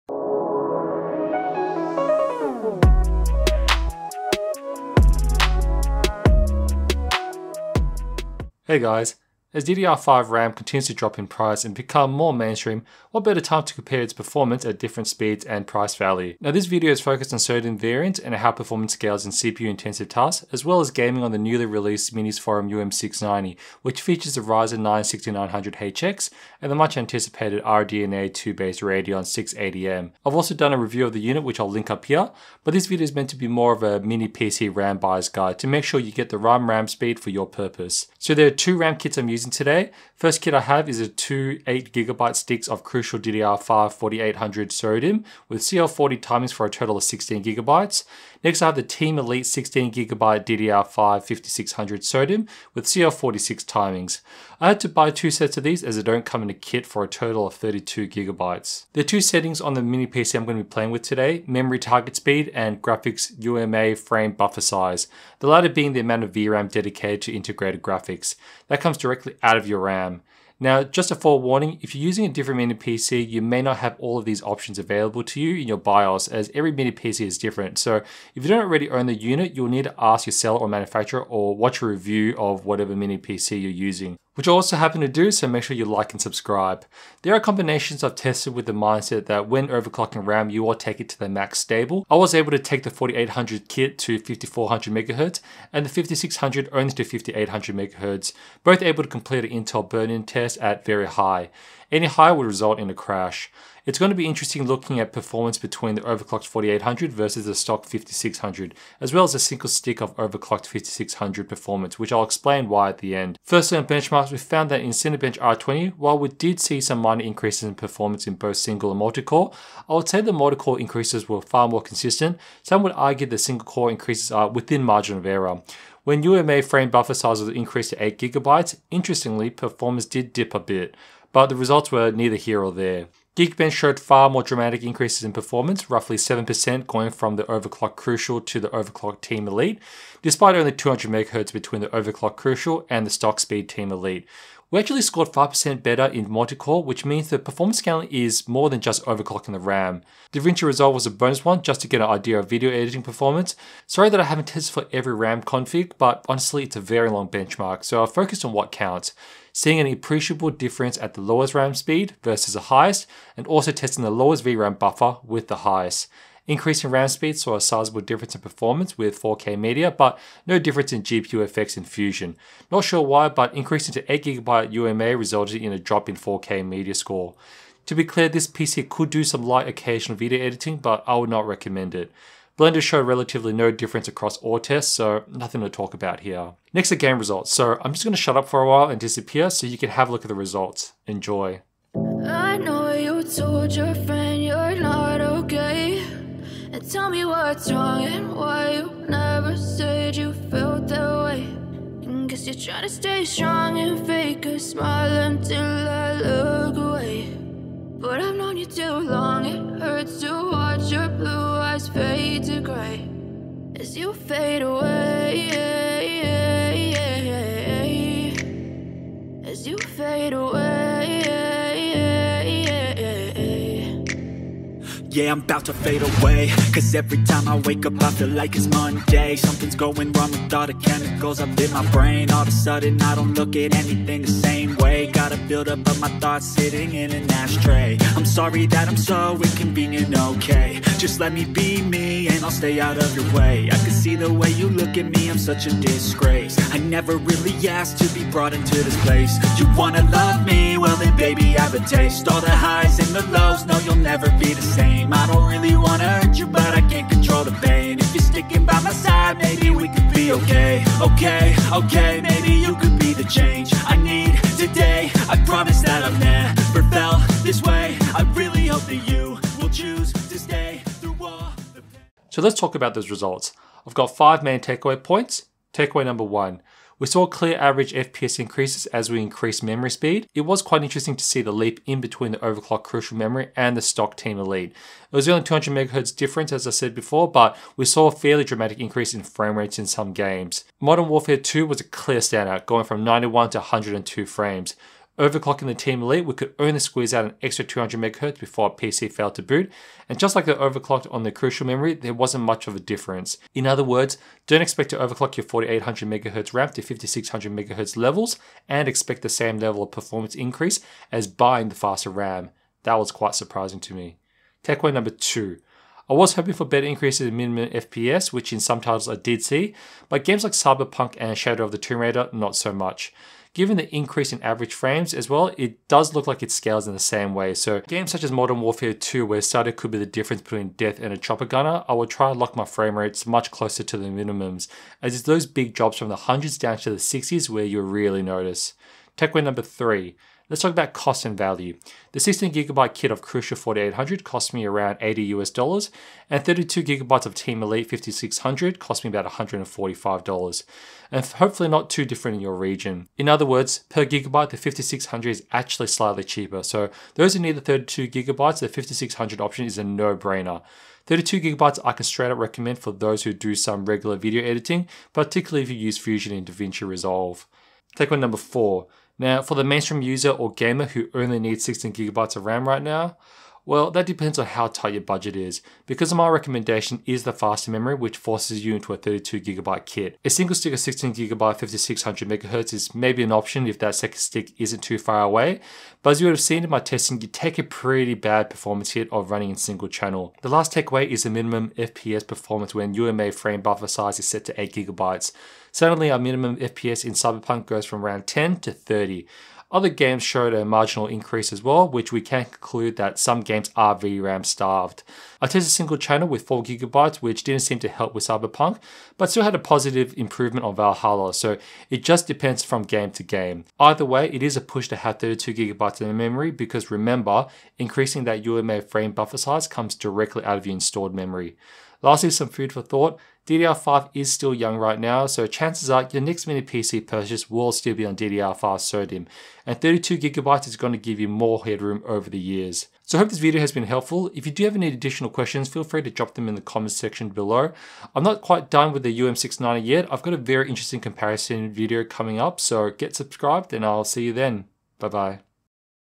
DDR5 SODIMM Memory Scaling 4800Mhz 5600Mhz Comparison Minisforum UM690 AMD 6900HX Benchmarks Gaming
Should you buy 4800Mhz or 5600Mhz DDR5 SODIMM? Today we explore how memory speed and VRAM scales in benchmarks and gaming. We've done our testing on the Minisforum UM690 with the Ryzen 6900HX and 680M Radeon Graphics.

Timecodes:
0:00 - Intro
0:39 - RAM kits, Testing method
1:53 - Synthetic Benchmarks
3:11 - Gaming Benchmarks
4:32 - Takeaway 1
5:15 - Takeaway 2
5:39 - Takeaway 3
6:07 - Takeaway 4
6:30 - Takeaway 5